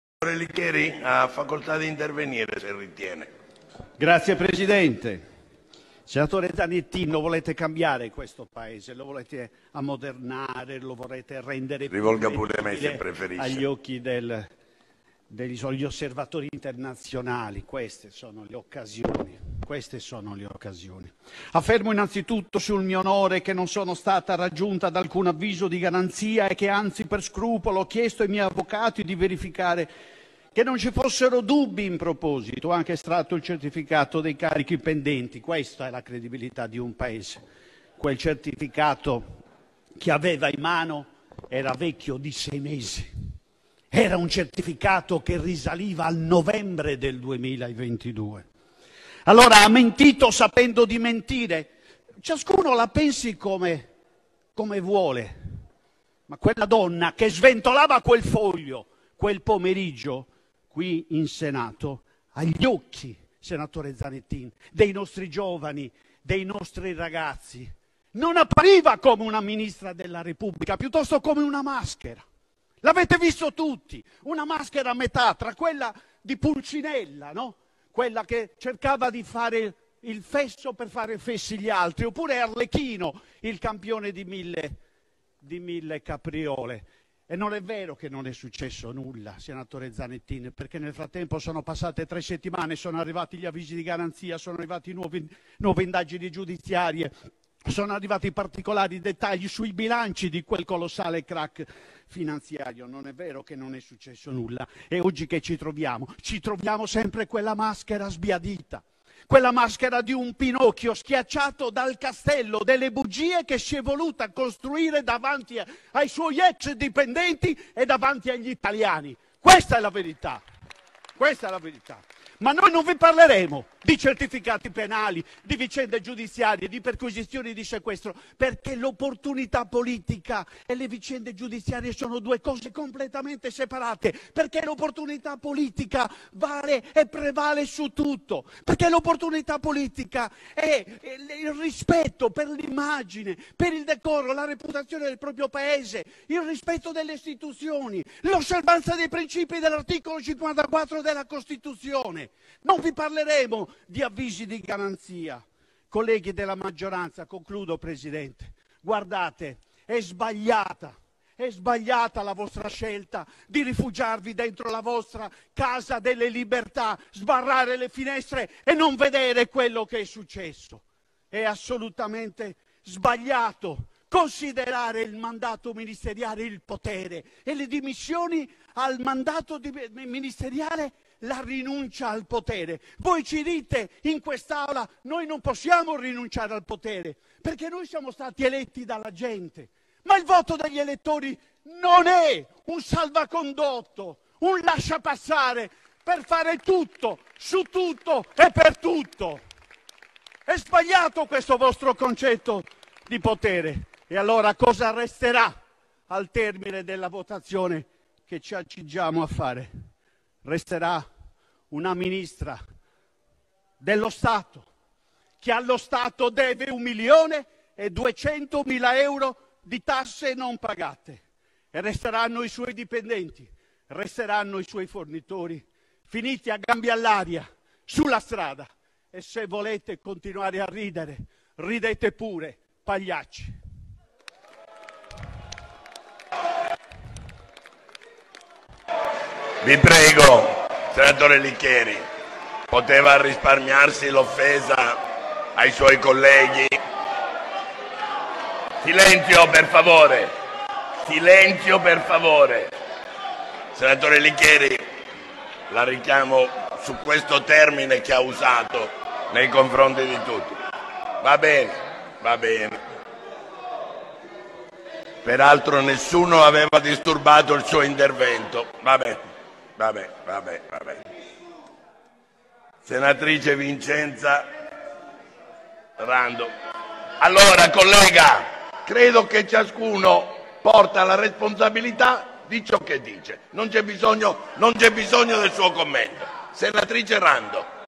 ETTORE LICHERI: "SANTANCHÉ DEVE 1 MILIONE E 200 MILA € DI TASSE ALLO STATO"
(Agenzia Italia News) IL SENATORE DEL MOVIMENTO 5 STELLE ETTORE LICHERI: "SANTANCHÉ DEVE 1 MILIONE E 200 MILA € DI TASSE ALLO STATO"